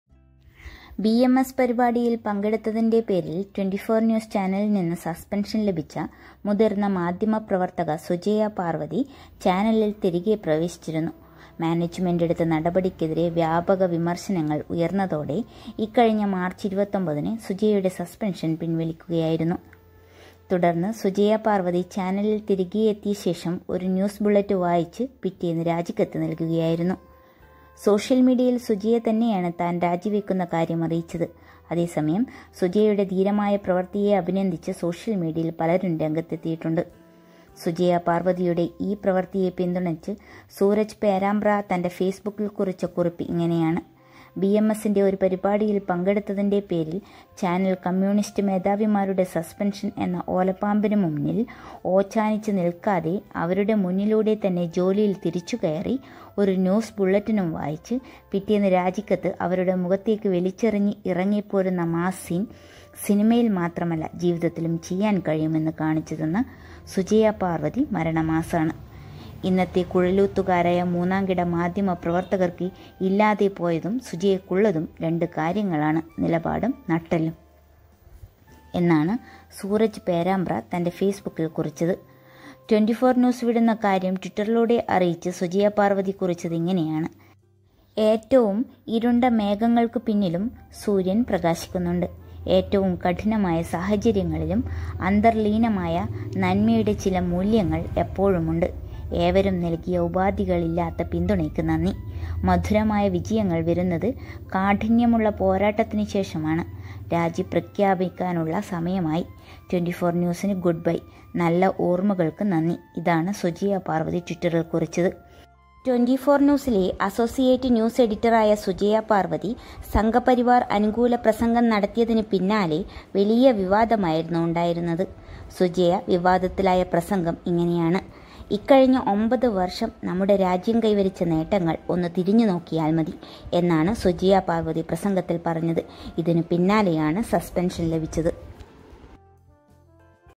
ഓലപ്പാമ്പിന് മുന്നിൽ ഓച്ചാനിച്ച് നിൽക്കാതെ, അവരുടെ മുന്നിലൂടെത്തന്നെ ജോലിയിൽ തിരിച്ചു കയറി...
ഓലപ്പാമ്പിന് മുന്നിൽ ഓച്ചാനിച്ച് നിൽക്കാതെ, അവരുടെ മുന്നിലൂടെത്തന്നെ ജോലിയിൽ തിരിച്ചു കയറി ഒരു ന്യുസ് ബുള്ളറ്റിനും വായിച്ച് പിറ്റേന്ന് രാജിക്കത്ത് അവരുടെ മുഖത്തേക്ക് വലിച്ചെറിഞ്ഞ് ഇറങ്ങി പോരുന്ന മാസ് സീൻ സിനിമയിൽ മാത്രമല്ല ജീവിതത്തിലും ചെയ്യാൻ കഴിയുമെന്ന് കാണിച്ചു തന്ന സുജയ പാർവ്വതി മര ണമാസ് ആണ്..കുറിപ്പ്

sujaya paravathy resigned from 24news